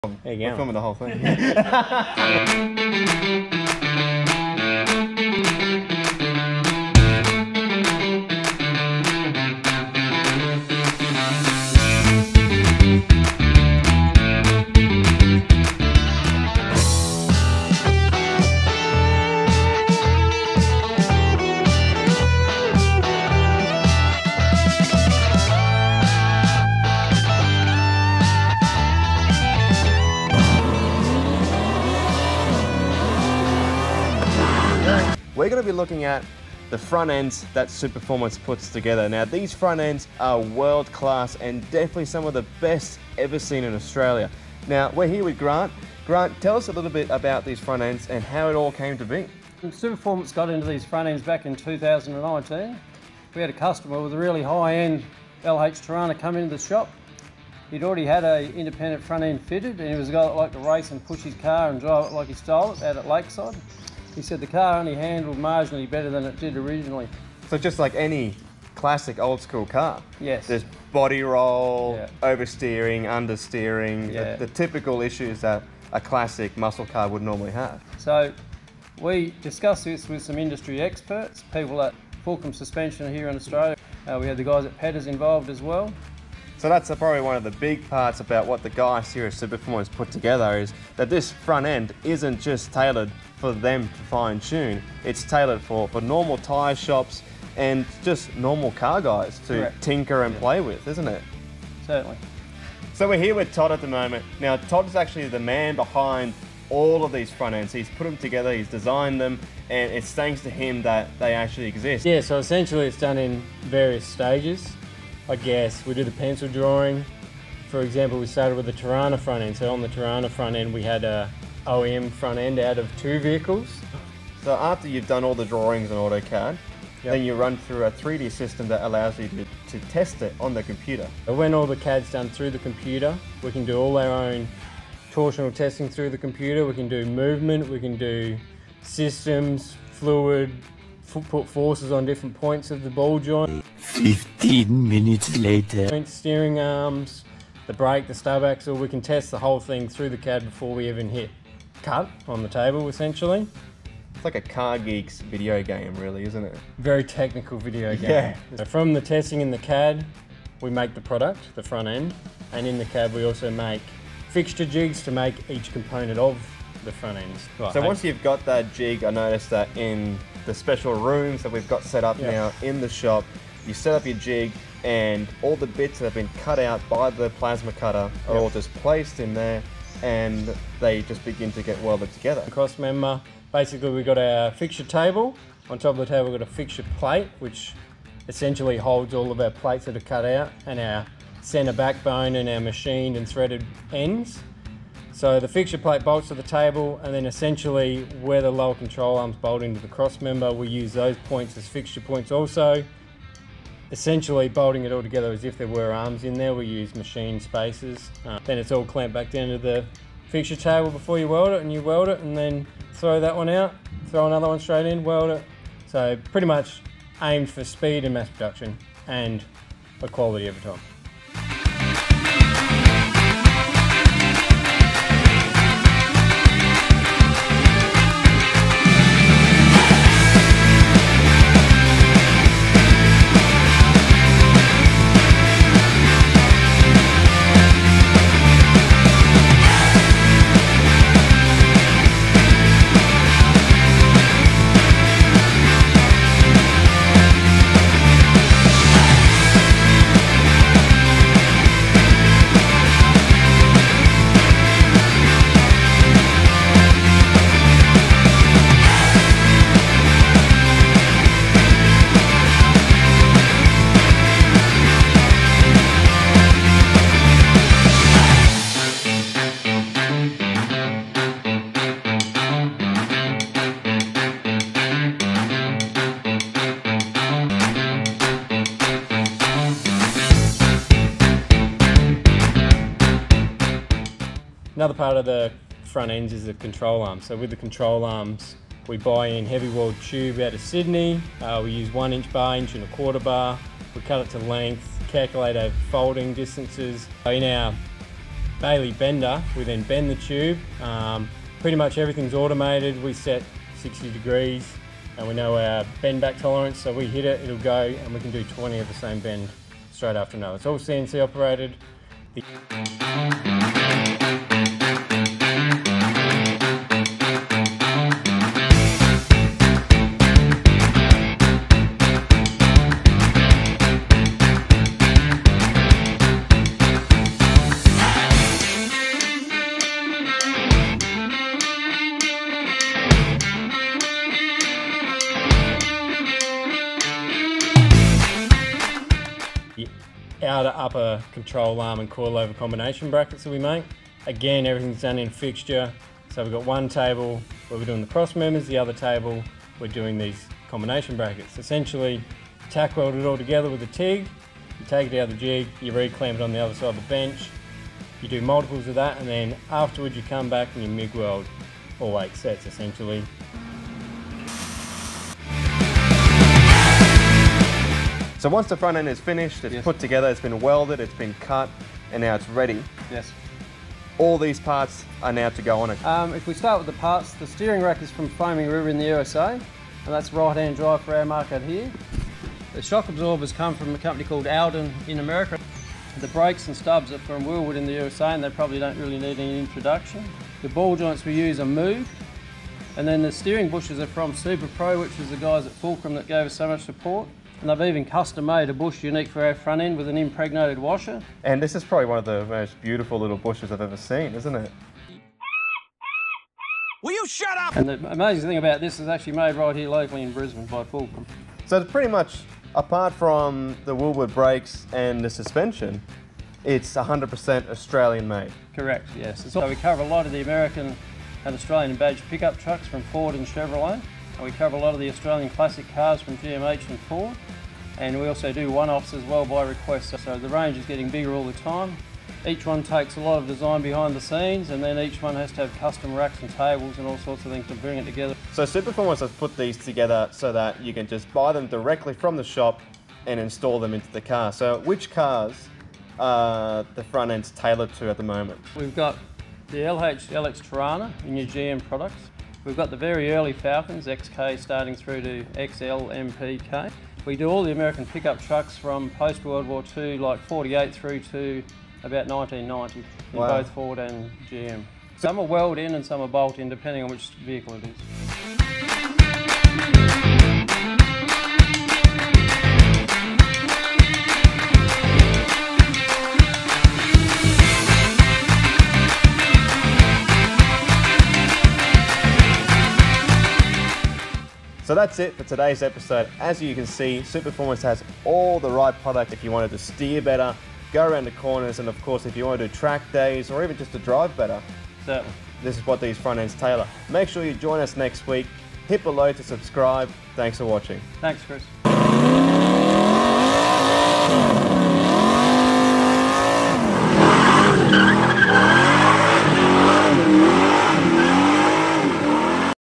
IFS - Independent Front Suspension Holden Ford Chevy
SUPERFORMANCE INHOUSE BUILT Independent Front Suspension Systems available for: Holden Torana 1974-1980 range LH, LX, UC. | Ford Pickup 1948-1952 | Ford F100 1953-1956 | Ford F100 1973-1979 | Chevrolet C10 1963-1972 | Chevrolet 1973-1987 & more to come!